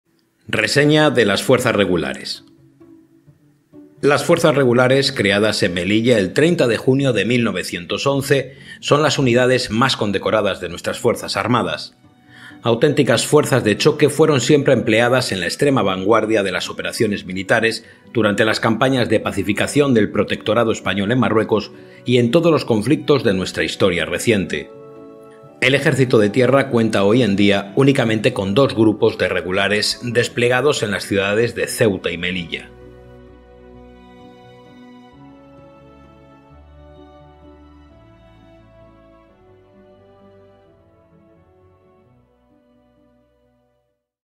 22TER004ESP RESEÑA DE LAS FUERZAS REGULARES
Las Fuerzas Regulares, creadas en Melilla el 30 de junio de 1911, son las unidades más condecoradas de nuestras Fuerzas Armadas. Auténticas fuerzas de choque, fueron siempre empleadas en la extrema vanguardia de las operaciones militares durante las campañas de pacificación del Protectorado español en Marruecos y en todos los conflictos de nuestra historia reciente. El Ejército de Tierra cuenta hoy en día únicamente con dos Grupos de Regulares desplegados en las ciudades de Ceuta y Melilla.